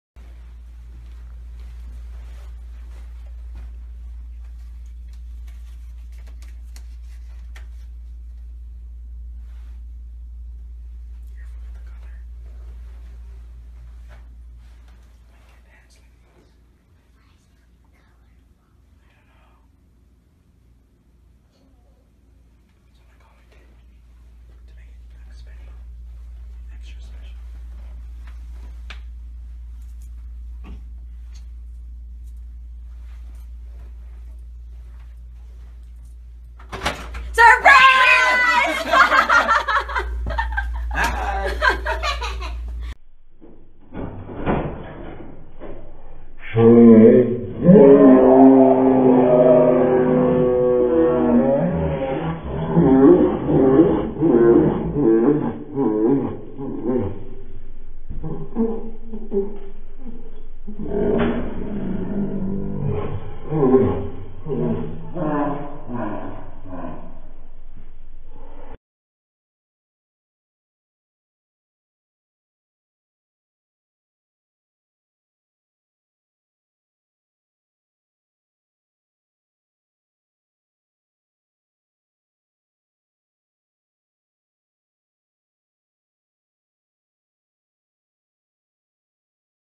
Paolo Ballesteros with daughter in America
Paolo Ballesteros Surprised reunion with Daughter Keira and former partner Katrina Nevada, in his hotel room in America with the help of Alden Richards. Is his boyfriend in the same room? Jose Manalo hinted in Juan for all, all for Juan, that Paolo is bringing his boyfriend to their US trip.